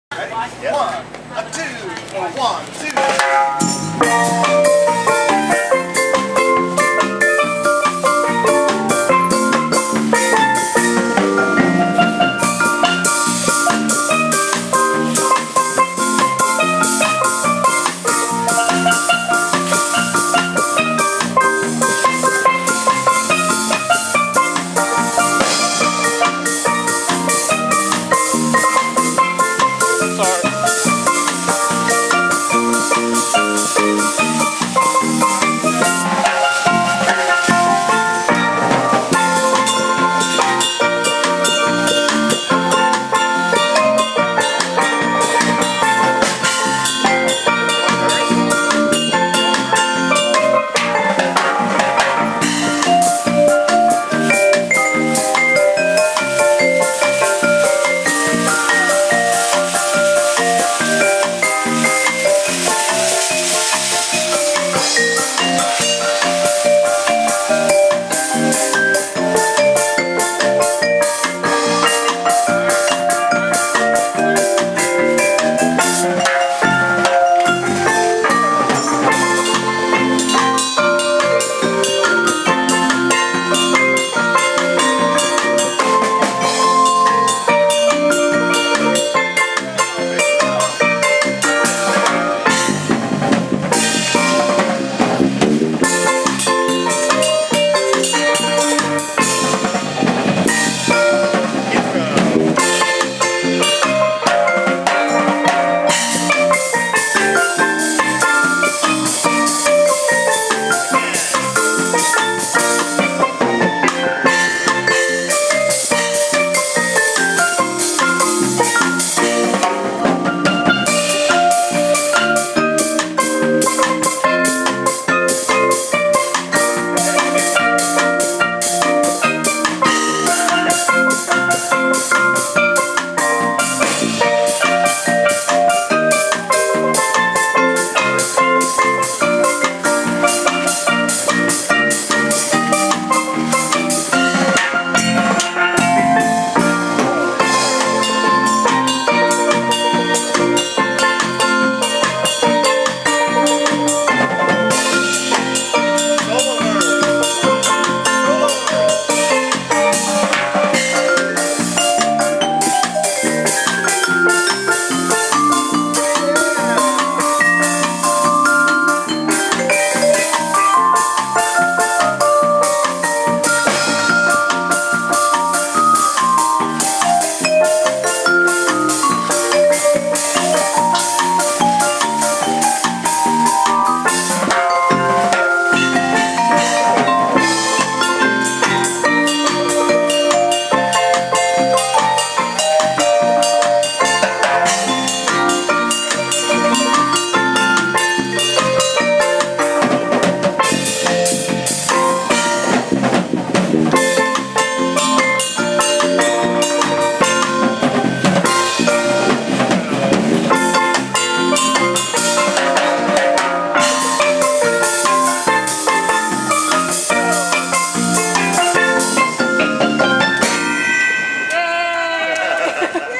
The Toucans play the Admiral Safeway grand opening
West Seattle Blog video of The Toucans performing at the grand opening of the new Admiral Safeway store on August 11, 2011.